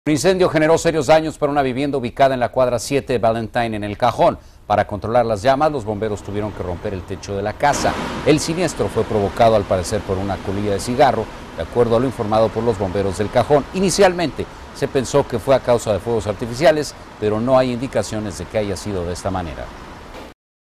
Incendio residencial en El Cajo
El Cajon - Un incendio genero daños para una vivienda ubicada en la cuadra 7 de Ballatyne en El Cajon.

Para controlar las llamas los bomberos tuvieron que romper el techo de la casa, el siniestro fue provocado al parecer por unas colillas de cigarro de acuerdo a lo informado por los bomberos de El Cajon.

Inicialmente se pensó que fue a causa de fuegos artificiales pero no hay indicaciones de que haya sido así.